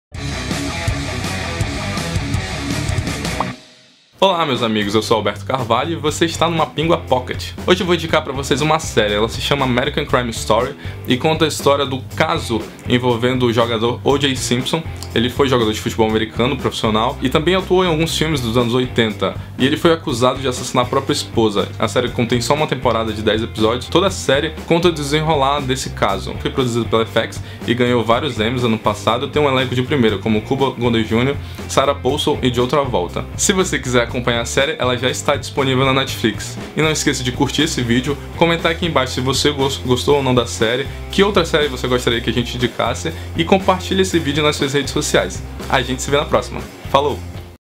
INDICAÇÃO: American Crime Story - MapinguaPOCKET
A antologia American Crime Story é a indicação. Nessa primeira temporada o foco é o caso O.J. Simpson, e mais 2 temporadas já foram confirmadas.